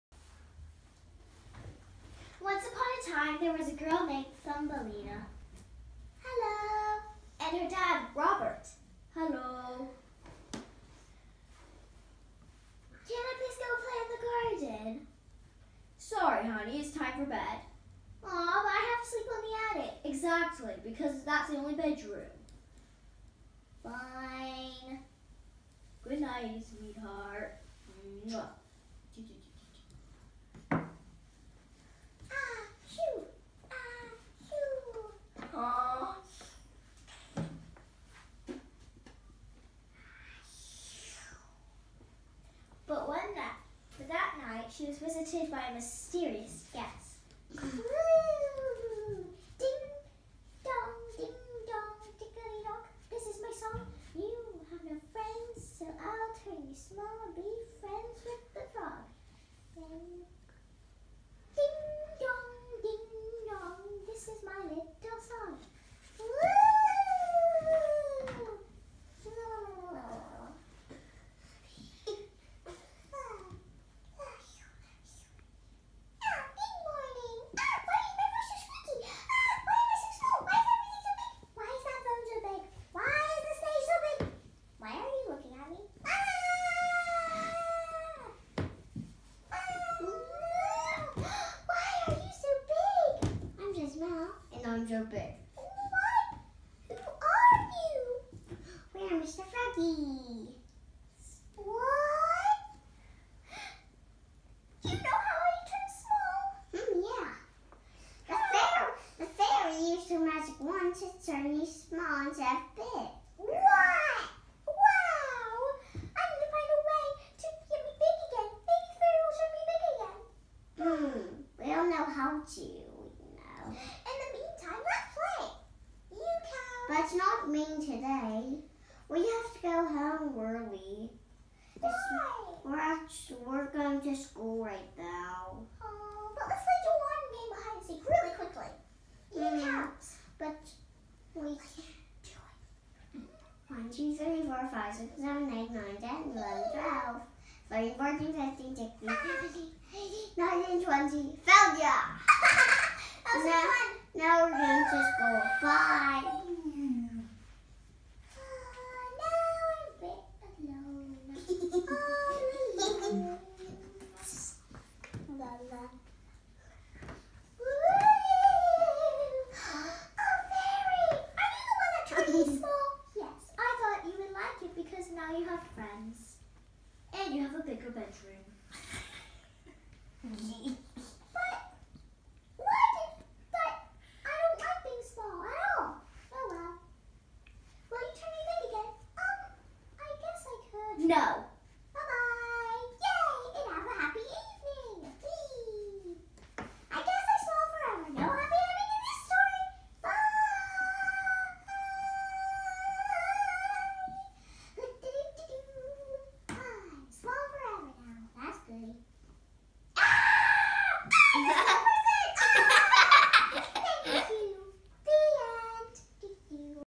Thumbalina 😁
Just some silliness from the B&L Puppet Theatre...